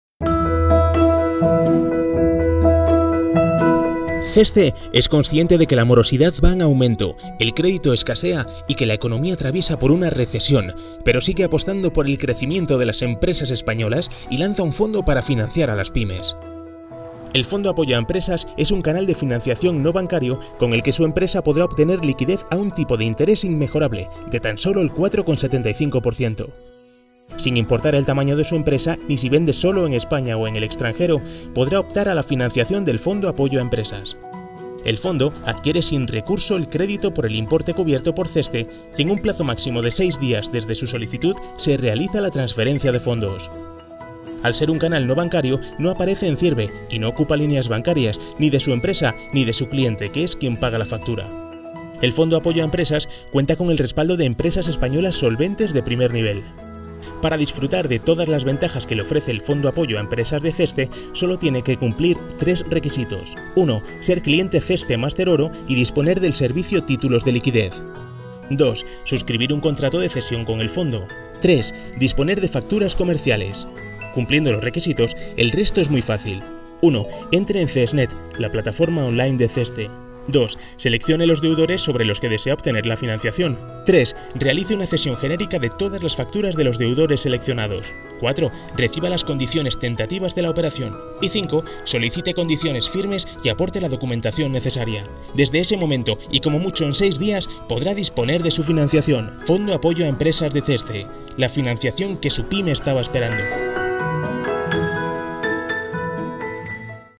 Fondo Apoyo a Empresas
Llega el Fondo Apoyo a Empresas, un canal de financiación no bancario que lanza CESCE. En este vídeo os contamos en qué consiste:

El Fondo Apoyo a Empresas es un canal de financiación no bancario con el que su empresa podrá obtener liquidez a un tipo de interés inmejorable, de tan sólo el 4,75%.
Sin importar el tamaño de su empresa, ni si vende sólo en España o en el extranjero, podrá optar a la financiación del Fondo Apoyo a Empresas.
El Fondo adquiere sin recurso el crédito por el importe cubierto por CESCE y en un plazo máximo de 6 días desde su solicitud, se realiza la transferencia de fondos.
Al ser un canal no bancario, no aparece en CIRBE y no ocupa líneas bancarias, ni de su empresa ni del proveedor que paga la factura
El Fondo Apoyo a Empresas cuenta con el respaldo de empresas españolas solventes de primer nivel.
Para disfrutar de todas las ventajas que le ofrece el Fondo Apoyo a Empresas de CESCE sólo tiene que cumplir tres requisitos: 1) Ser cliente CESCE MASTER ORO y disponer del servicio Títulos de Liquidez. 3) Disponer de facturas comerciales.

Cumpliendo los requisitos, el resto es muy fácil:
1) Entre en CESNET, la plataforma online de CESCE. 2)Seleccione los deudores sobre los que desea obtener la financiación. 3)Realice una cesión genérica de todas las facturas de los deudores seleccionados. 4)Reciba las condiciones tentativas de la operación. 5)Solicite condiciones firmes y aporte la documentación necesaria.
Desde ese momento, y como mucho en 6 días, podrá disponer de su financiación.

Fondo Apoyo a Empresas de CESCE. La financiación que su PYME estaba esperando.